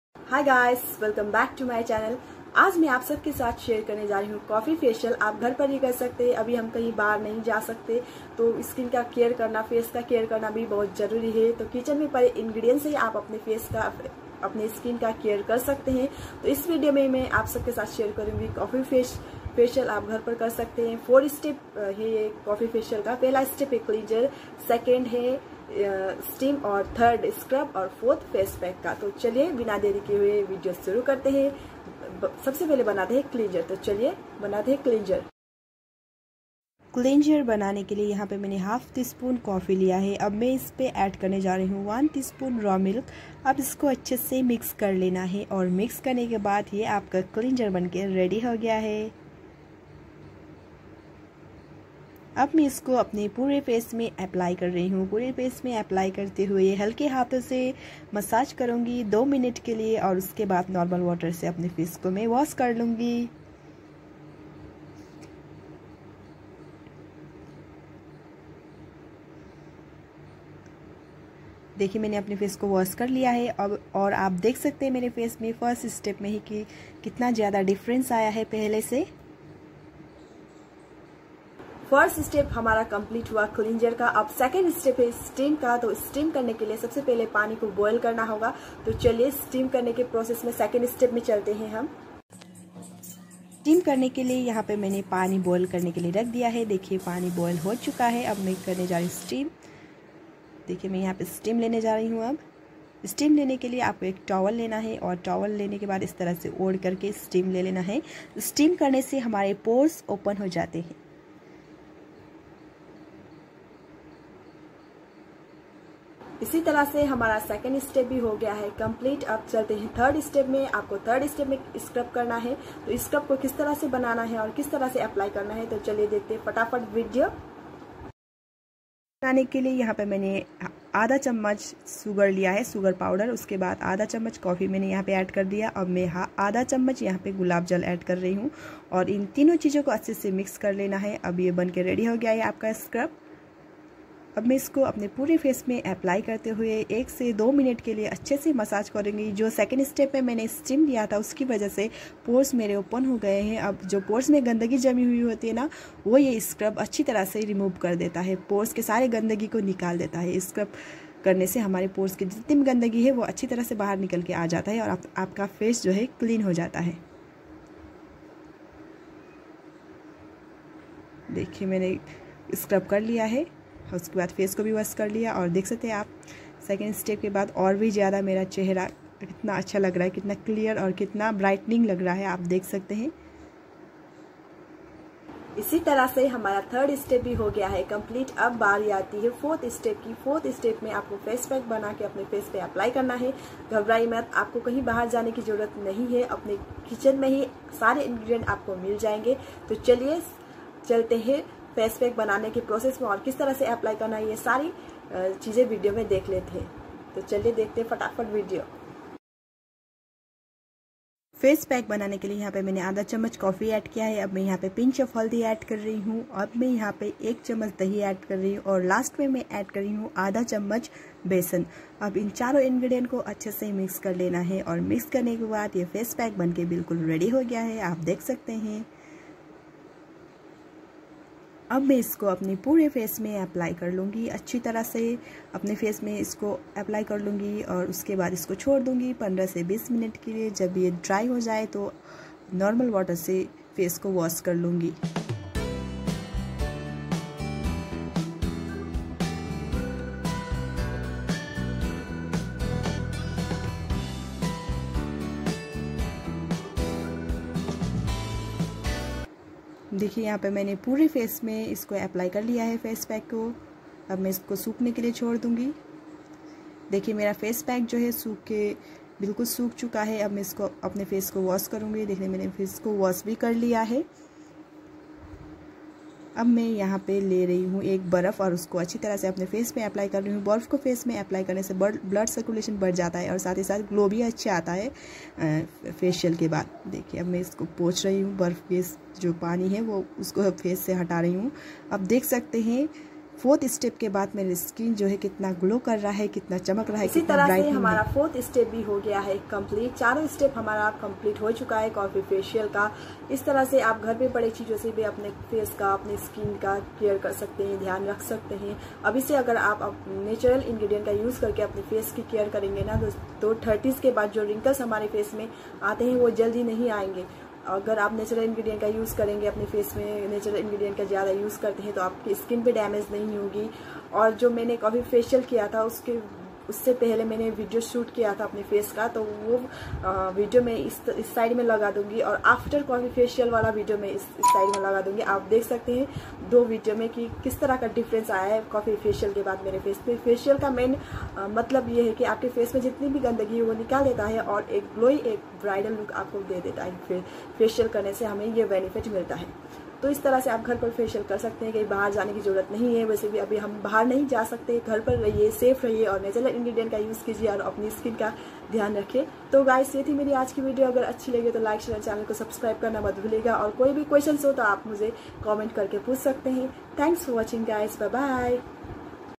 Skin Brightening coffee facial at home||Apnea chehre ko chamkaye coffee facial se ghar per
Brightening coffee facial at home
Apnea chehre ko chamkaye coffee facial se ghar per